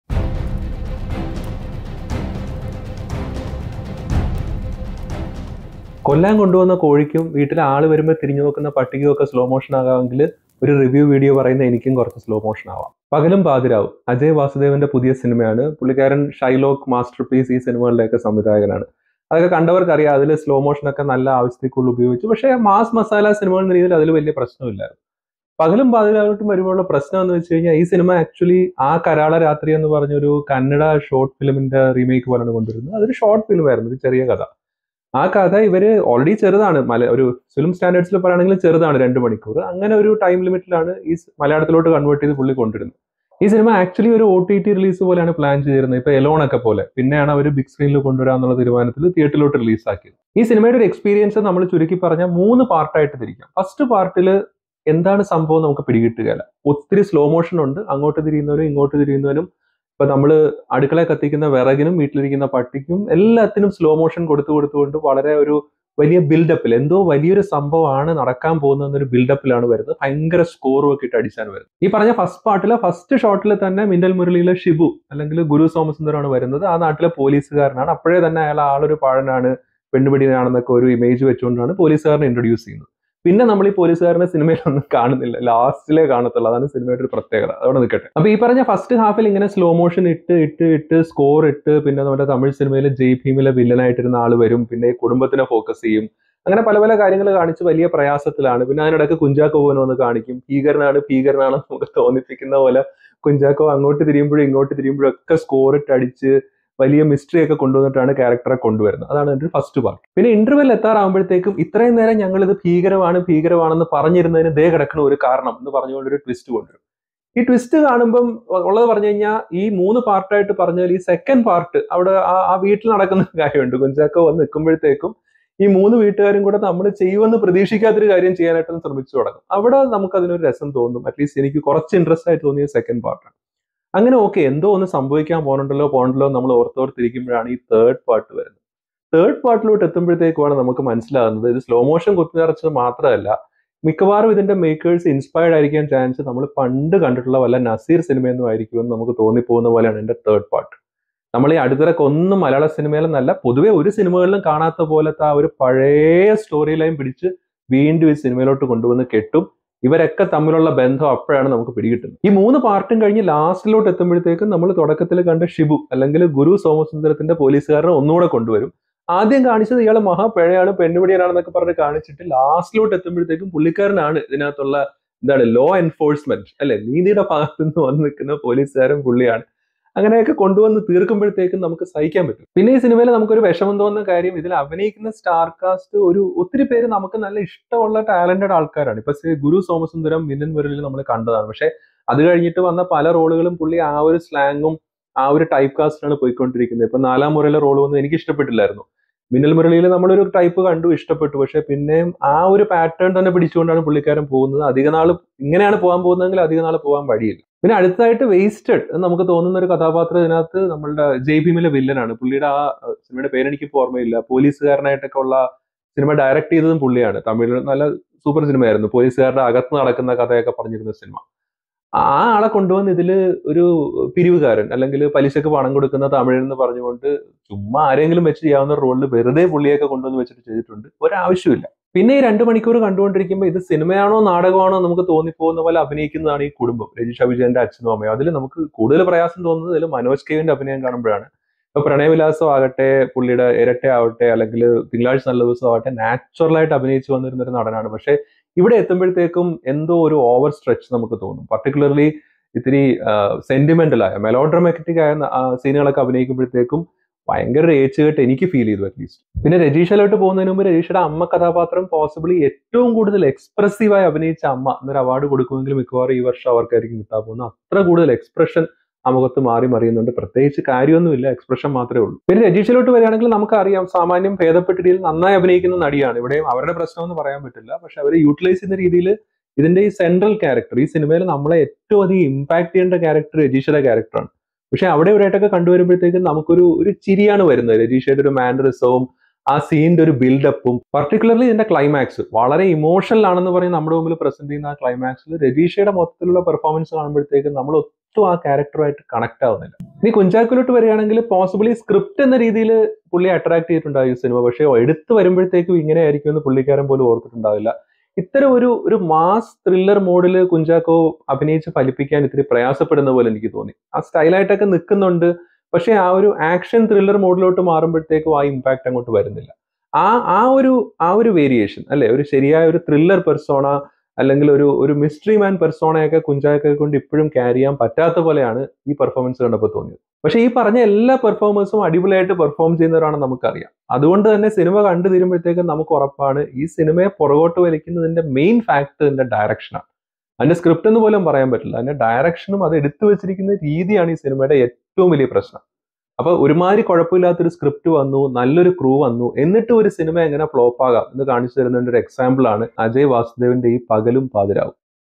Pakalum Pathiravum Movie Review Malayalam| Slow Motion out of control
My opinion on the malayalam movie Pakalum Pathiravum  directed by Ajaya Vasudev starring Kunchacko Boban, Rajisha Vijayan, Guru Somasundaram